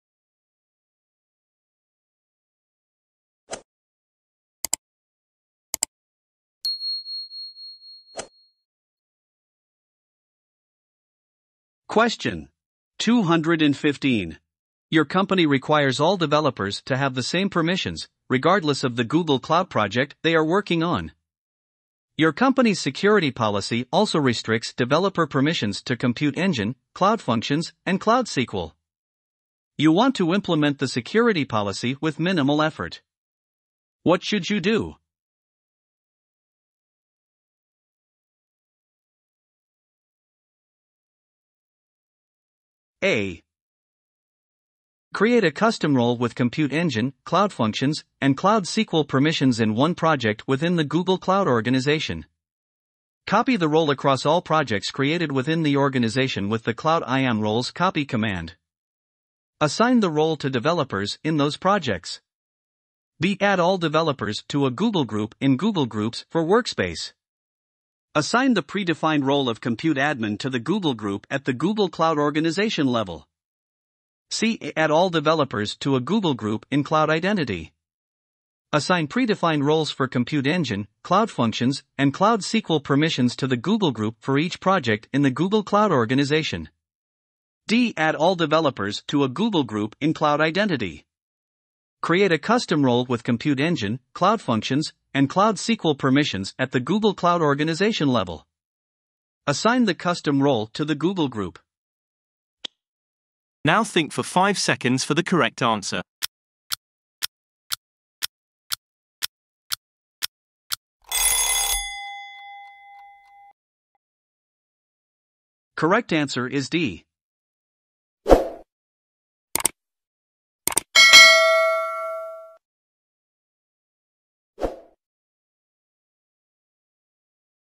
Q215. Your company requires all developers to have the same permissions, regardless of the Google
Q215. Your company requires all developers to have the same permissions, regardless of the Google Cloud project they are working on. Your company’s security policy also restricts developer permissions to Compute Engine, Cloud Functions, and Cloud SQL. You want to implement the security policy with minimal effort. What should you do?​

A. • Create a custom role with Compute Engine, Cloud Functions, and Cloud SQL permissions in one project within the Google Cloud organization. ​
• Copy the role across all projects created within the organization with the gcloud iam roles copy command.​
• Assign the role to developers in those projects.​

B. • Add all developers to a Google group in Google Groups for Workspace.​
• Assign the predefined role of Compute Admin to the Google group at the Google Cloud organization level.​

C. • Add all developers to a Google group in Cloud Identity. ​
• Assign predefined roles for Compute Engine, Cloud Functions, and Cloud SQL permissions to the Google group for each project in the Google Cloud organization.​

D. • Add all developers to a Google group in Cloud Identity. ​
• Create a custom role with Compute Engine, Cloud Functions, and Cloud SQL permissions at the Google Cloud organization level.​
• Assign the custom role to the Google group.